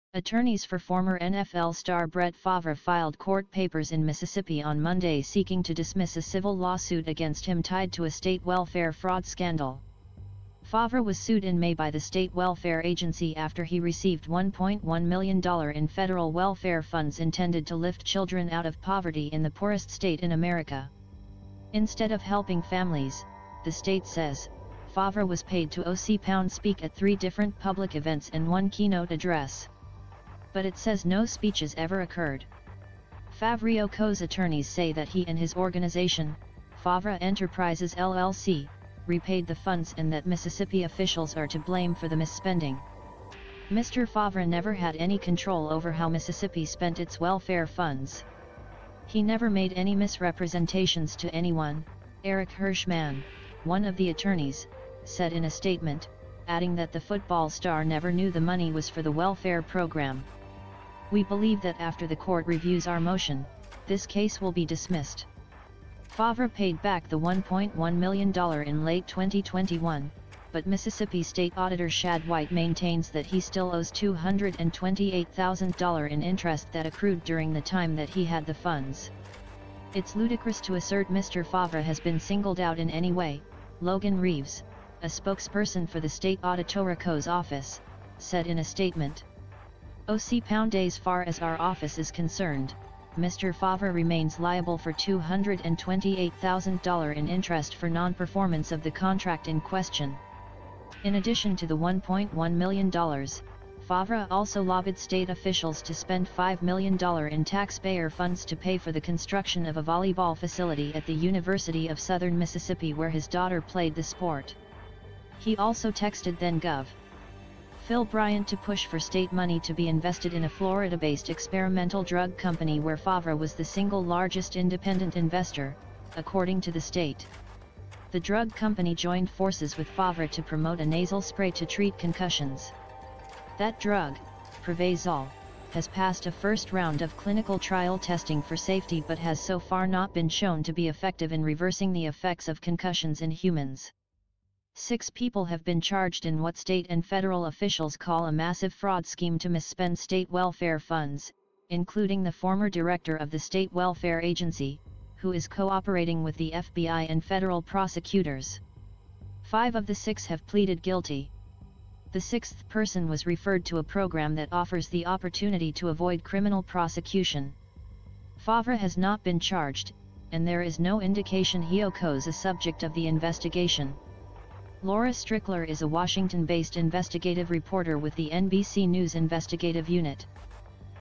Brett Favre files motion to dismiss lawsuit in Mississippi welfare fraud case
Attorneys for former NFL star Brett Favre filed court papers in Mississippi Monday seeking to dismiss a civil lawsuit against him tied to a state welfare fraud scandal.

Attorneys for former NFL star Brett Favre filed court papers in Mississippi on Monday seeking to dismiss a civil lawsuit against him tied to a state welfare fraud scandal. Favre was sued in May by the state welfare agency after he received $1.1 million in federal welfare funds intended to lift children out of poverty in the poorest state in America. Instead of helping families, the state says, Favre was paid to “speak at three different public events and one keynote address.”